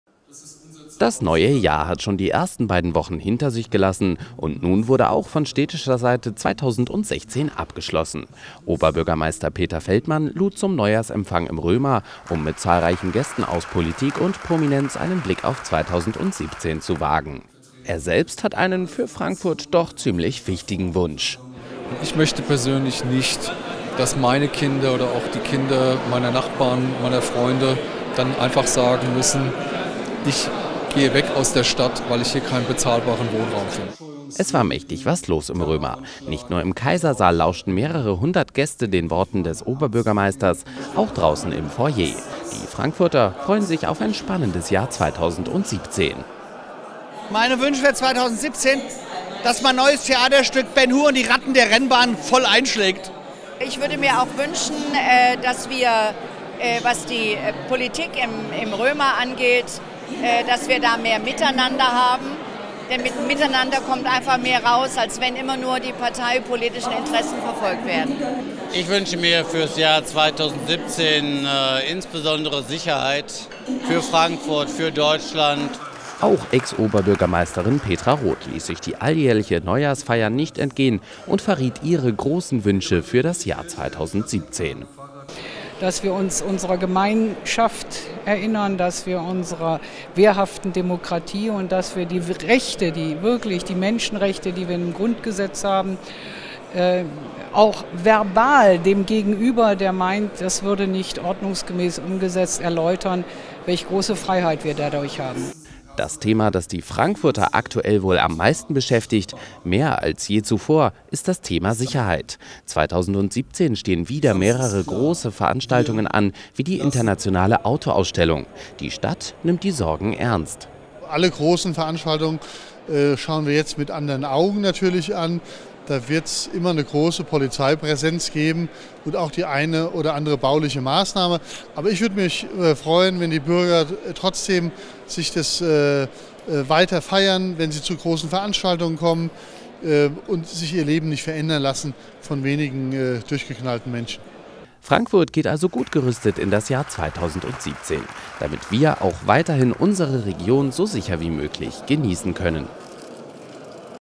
Neujahrsempfang in Frankfurt
Der Neujahrsempfang im Römer ist Tradition. Auch wir haben uns dieser Tradition nicht entzogen und zeigen Ihnen, was sich die Frankfurter für 2017 wünschen.